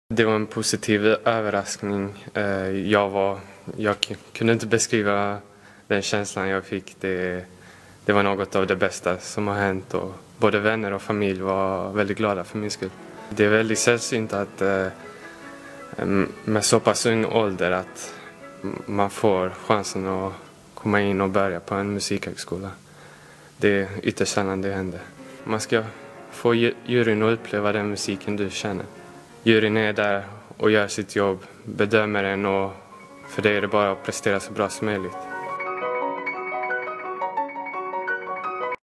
Från krossade pianistdrömmar till Köpenhamn
Hans drömmar om att bli konsertpianist vara nära att krossas av byråkrati för några år sen, när han inte fick fortsätta på synkopen för att han bodde i Bjuv.

Publicerad på hd.se söndag 5 maj 2013.